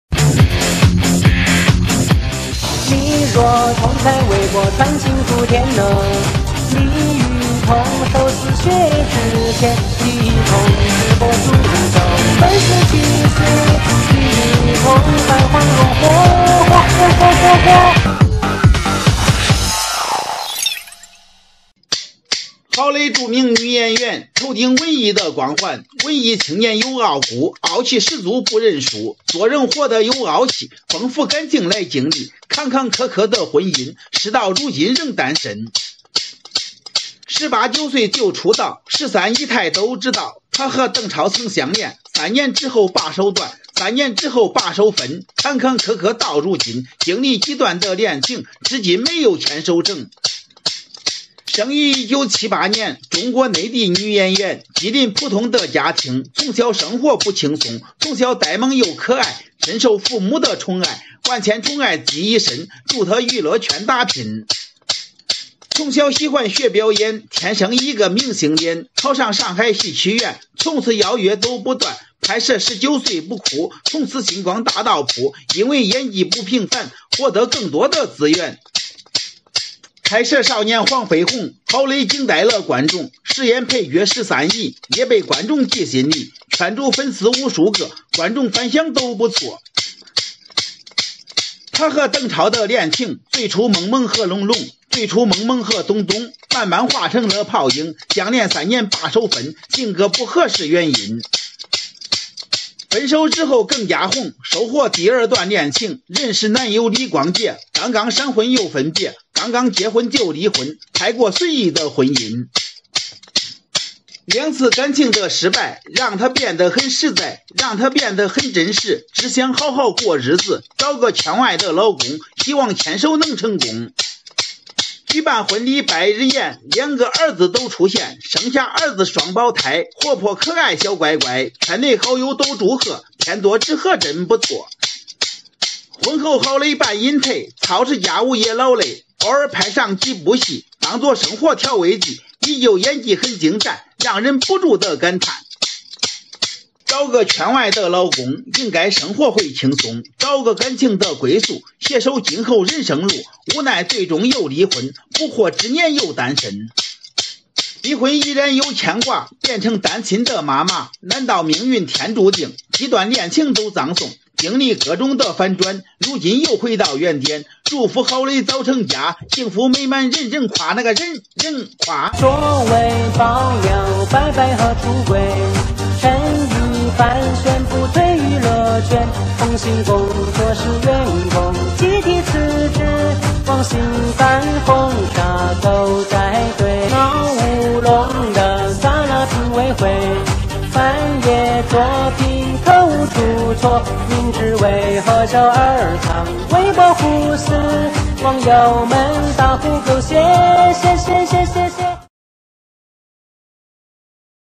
歪歌社团 娱事我知道：“十三姨”郝蕾的感情史
郝蕾著名女演员，头顶文艺的光环。文艺青年有傲骨，傲气十足不认输。做人活的有傲气，丰富感情来经历。坎坎坷坷的婚姻，事到如今仍单身……